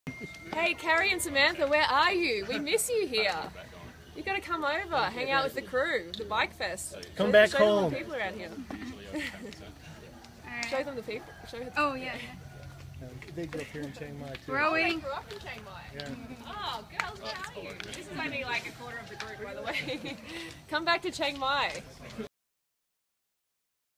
From Freelee the Banana Girl: Hay Kerry and Samantha!
A short message for my daughters from @FreeleeBG Freelee the Banana Girl.